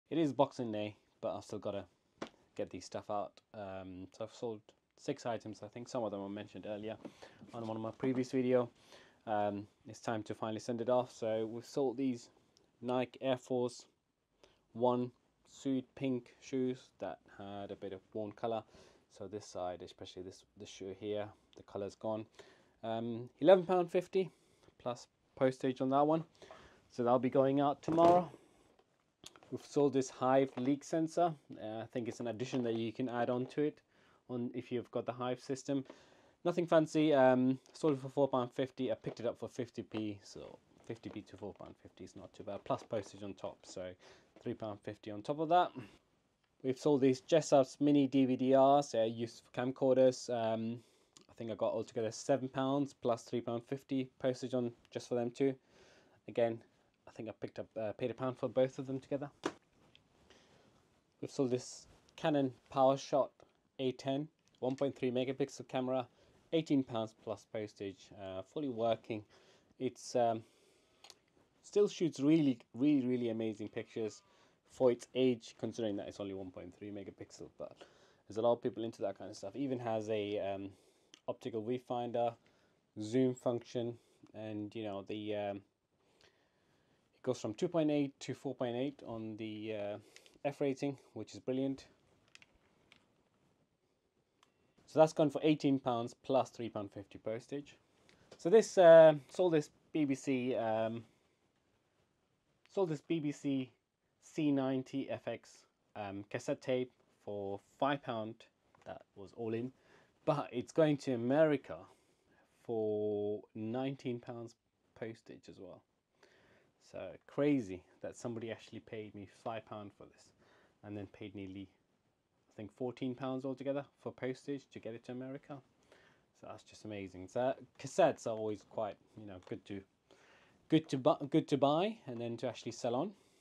shortest sales update of the year: UK eBay Reseller
Quick shipping update for boxing day. A buyer bought a £5 tape and paid £14.78 to ship it to the US.

Feedback is greatly appreciated!